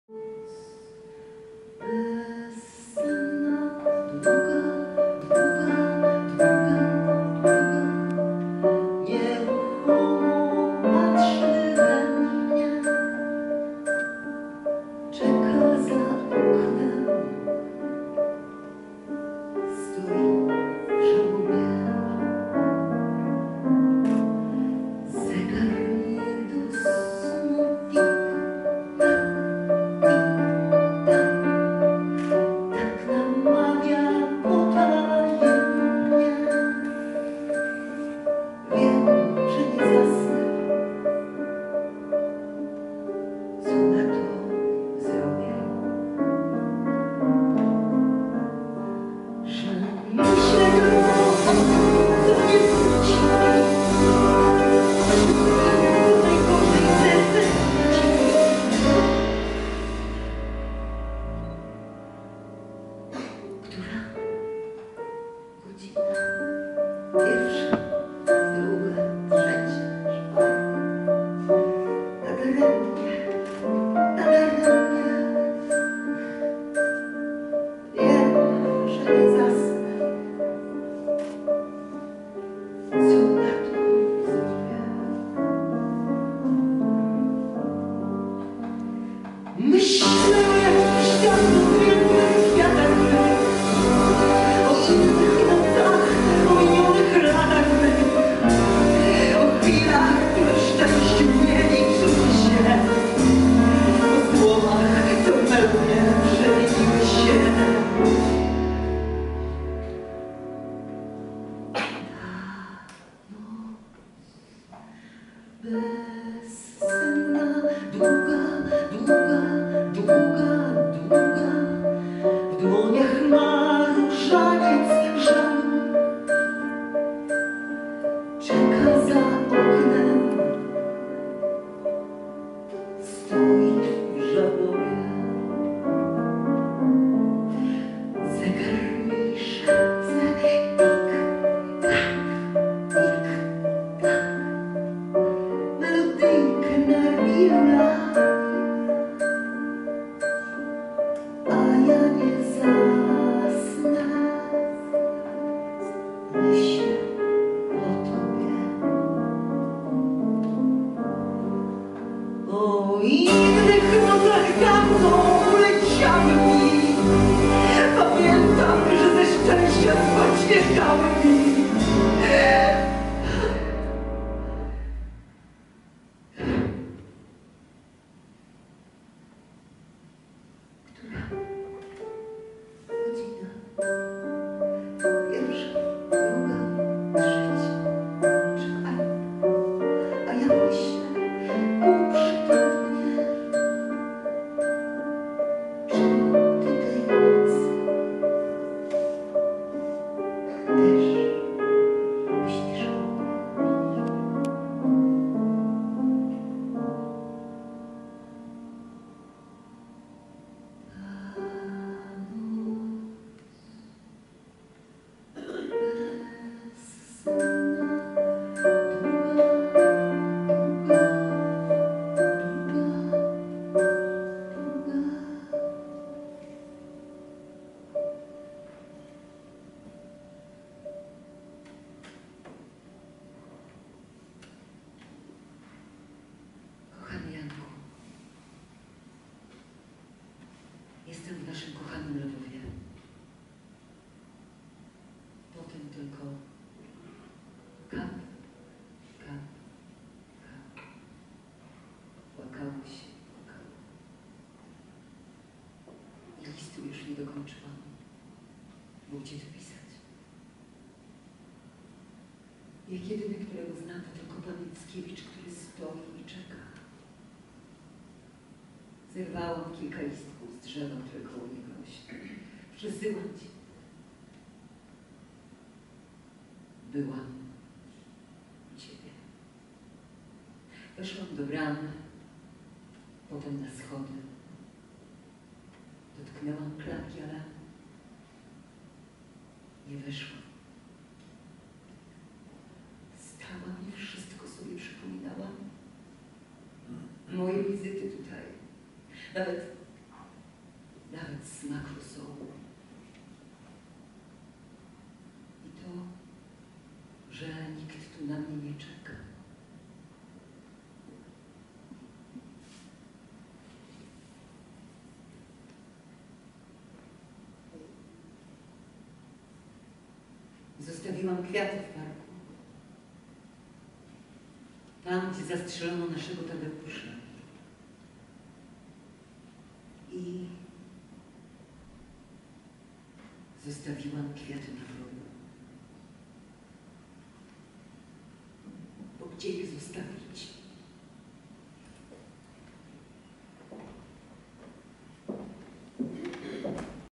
Spektakl M. Zimińska
W wykonaniu: Magdalena Tomaszewska oraz zespół instrumentalny w składzie: fortepian - Edward Bogdan, kontrabas - Tomasz Krawczyk, perkusja - Jakub Krawczyk. Spektakl odbył się w dniu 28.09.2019 - Muzeum Romantyzmu w Opinogórze.